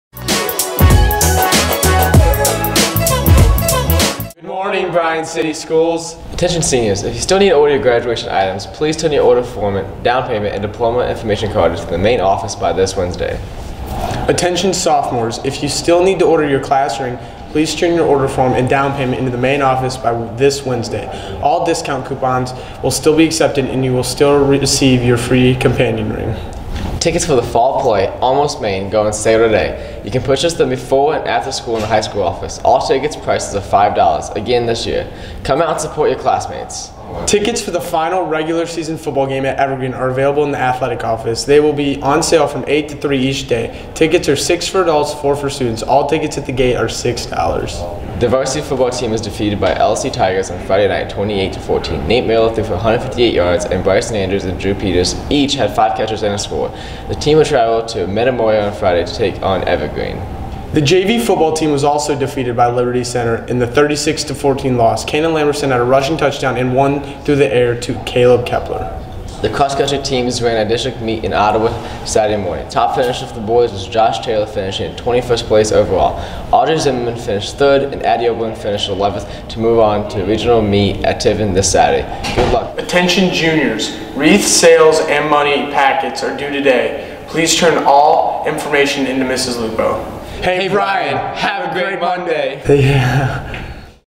Announcements - 10/22/18
Tickets for the fall play, ALMOST, MAINE go on sale today.  You can purchase them before and after school in the high school office.  All ticket prices are $5 again this year. Come out and support your classmates. (11/9)

Tickets for the final regular season football game at Evergreen are available in the athletic office. They will be on sale from 8:00 to 3:00 each day. Ticket prices are $6.00 for adults, $4.00 for students. All tickets at the gate are $6.00.

The varsity football team was defeated by the LC Tigers on Friday night, 28 to 14. Nate Miller threw for 158 yards and Brycen Andrews and Drew Peters each had 5 catches and a score. The team will travel to Metamora on Friday to take on Evergreen.

The JV football team was also defeated by Liberty Center. In the 36 to 14 loss, Canon Lamberson had a rushing touchdown and one through the air to Caleb Kepler.

The cross country teams ran at the district meet in Ottawa Saturday morning. Top finisher for the boys was Josh Taylor finishing in 21st place overall. Audrey Zimmerman finished 3rd and Addie Oberlin finished 11th to move on to the regional meet at Tiffin this Saturday. Good luck, ladies!

All wreath orders are due today to Mrs. Lupo!